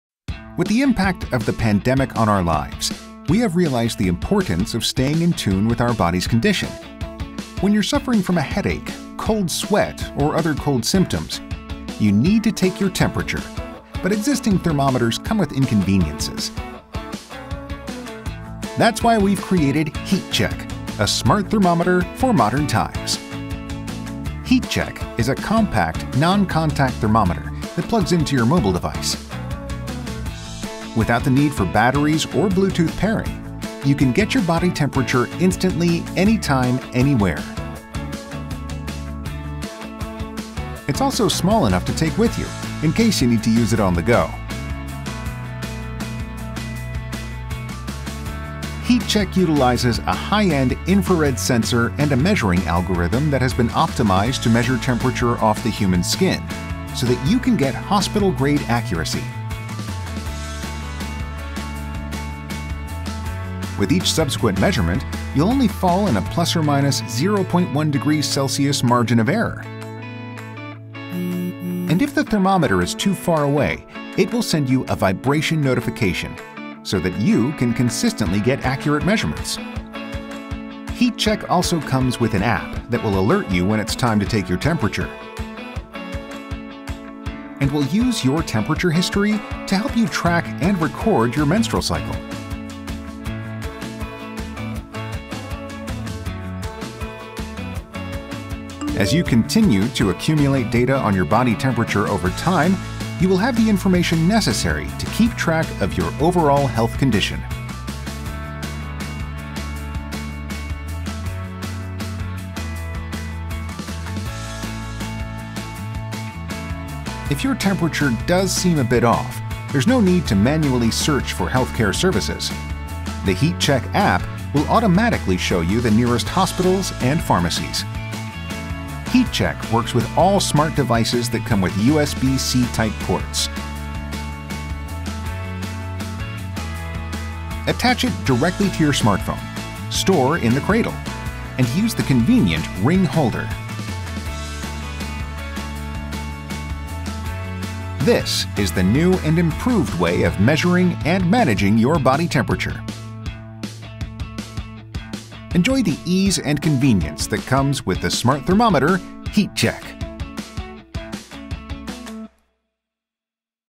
CES 2024 Innovation Awards Honoree | HiiTCHECK
Say hello to HIITCHECK,
your partner that will always be available to help you stay informed of your daily health & condition.
Add health to your daily routine, with HiiTCHECK!

[ CES 2024 | 디지털 헬스 혁신상 ]
정확성, 편리함, 감성까지 모두 담은 배터리 없는 피부 적외선 체온계 HiiTCHECK은 2초면 됩니다.

+ 배터리가 없는 비접촉 스마트 체온계 “HiiTCHECK(힛첵)”은 크기가 작고 휴대가 용이하며, 스마트폰 거치대 타입 액세사리 형태로 부착하여 다니실 수 있으며, 체온 측정이 필요한 경우 C-Type 스마트폰 충전구에 꽂아 사용이 가능한 제품입니다. 별도의 배터리, 블루투스나 와이파이 등의 연결 없이 스마트폰과 애플리케이션만 있으면 언제 어디서나 간편하게 측정하고 자동으로 체온 데이터를 기록해 사용자의 기초 체온을 체크할 수 있습니다.
본 제품은 2등급 의료기기인 피부적외선 체온계 입니다.